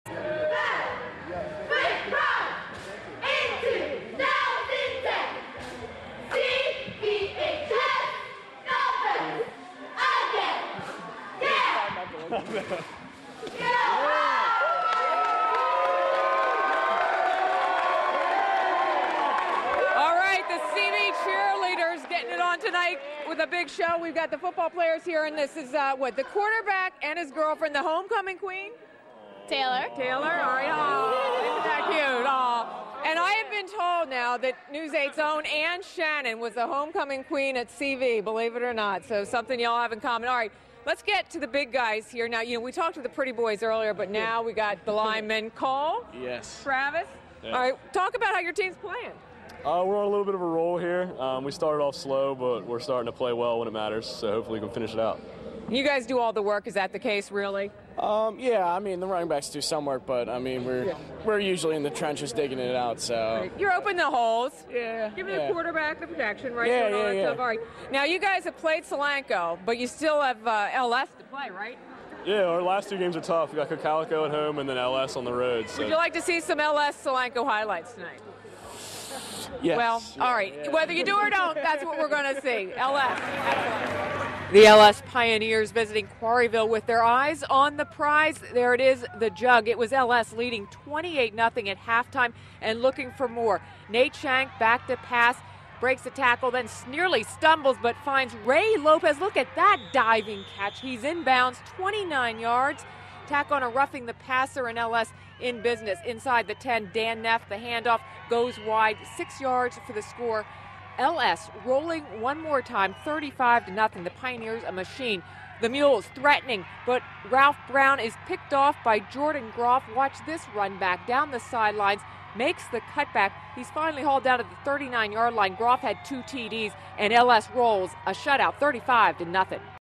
Lampeter-Strasburg Takes Home 'The Jug'
The Pioneers stuffed Solanco 35-0.